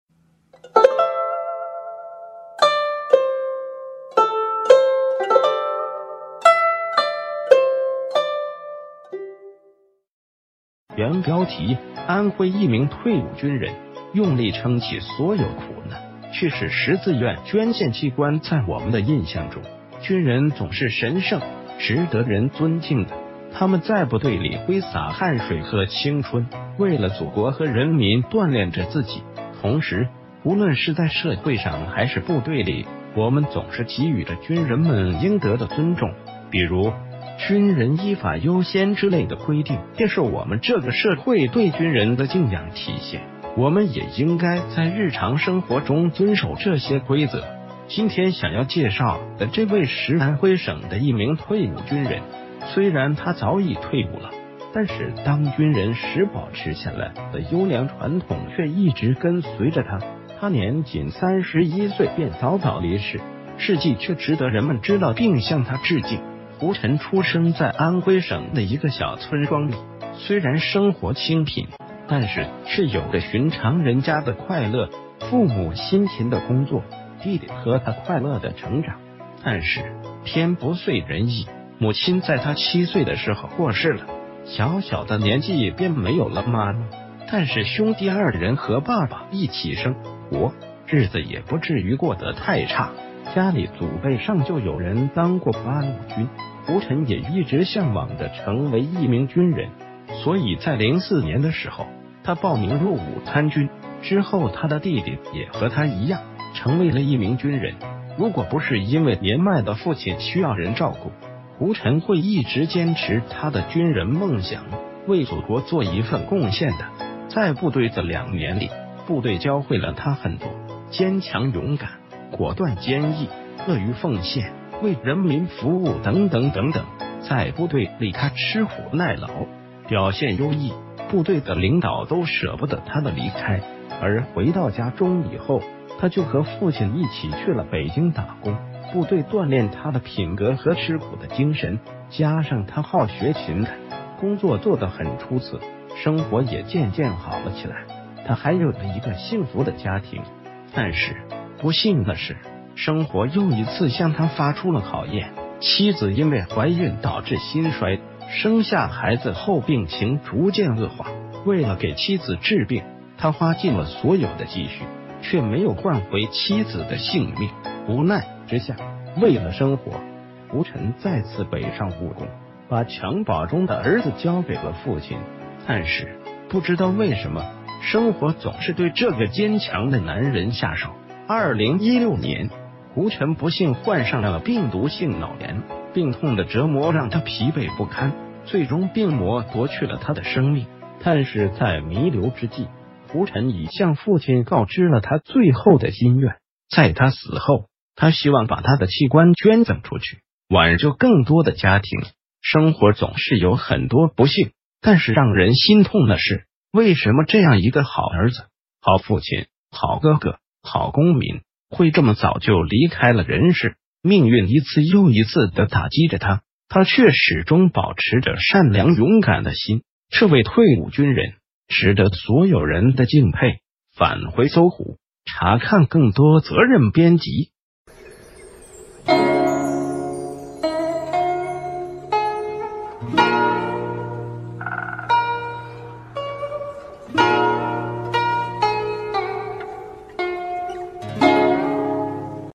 安徽一名退伍軍人，用力撐起所有苦難，去世時自愿捐獻器官_搜狐歷史_搜狐網
原標題：安徽一名退伍...